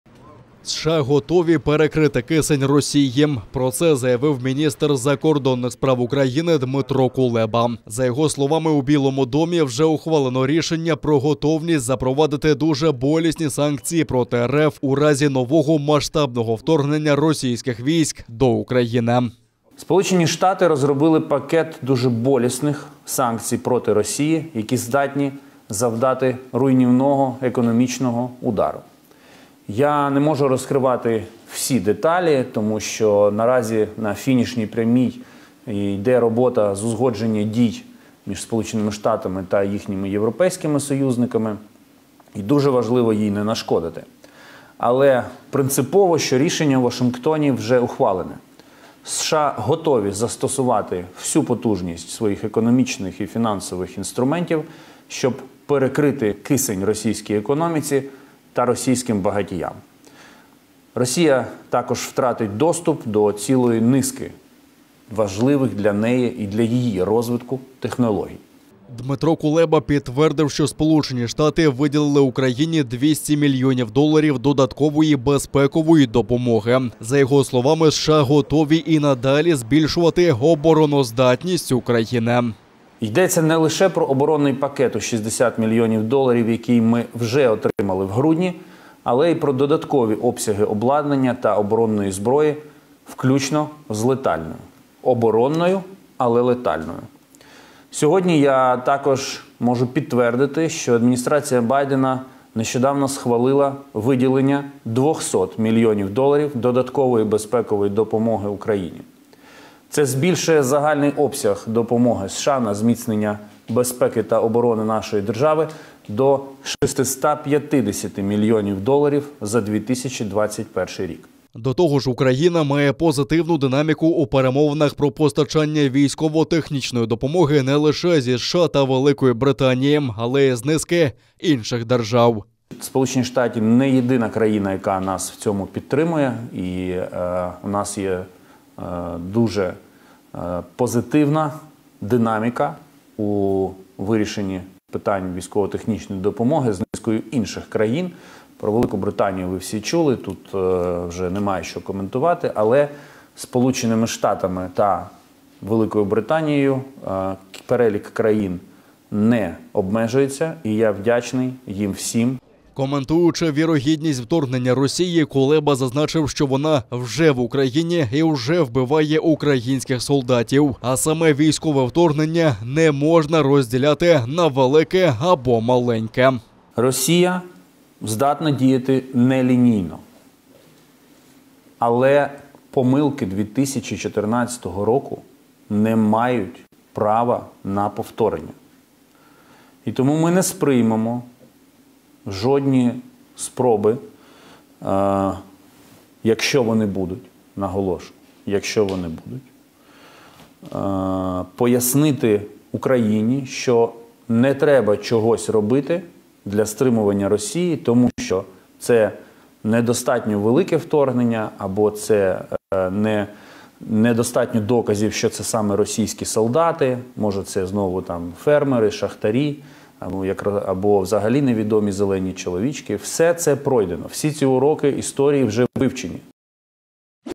Рішення вже ухвалене: Кулеба заявив про готовність США "перекрити кисень" Росії
Міністр закордонних справ Дмитро Кулеба заявив, що державний секретар США Ентоні Блінкен особисто запевнив його у готовності США "перекрити кисень" російській економіці та російським багатіям у разі вторгнення в Україну.
Про це Кулеба сказав на онлайн-брифінгу в четвер, 20 січня, повідомляє кореспондент Укрінформу.

«США розробили пакет дуже болісних санкцій проти Росії, які здатні завдати руйнівного економічного удару. Я не можу розкривати всі деталі, тому що наразі на фінішній прямій робота з узгодження дій між США та їхніми європейськими союзниками, і дуже важливо їй не нашкодити. Але принципово, що рішення у Вашингтоні вже ухвалене: США готові застосувати всю потужність своїх економічних і фінансових інструментів, щоб перекрити кисень російській економіці та російським багатіям. Росія також втратить доступ до цілої низки важливих для її розвитку технологій», - сказав Кулеба.

Він додав, що вчора держсекретар Ентоні Блінкен особисто запевнив у готовності США застосувати ці та інші важелі для стримування Росії.